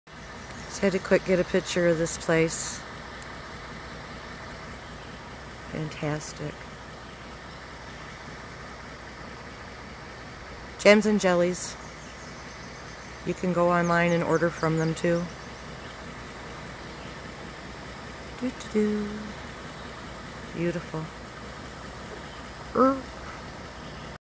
The Jam Pot
Poorrock Abbey  located on M26 Eagle Harbor Michigan  Jams, Jelly,  homemade Carmel,  Brownies and many delicious treats.   Poorrock Abbey. Com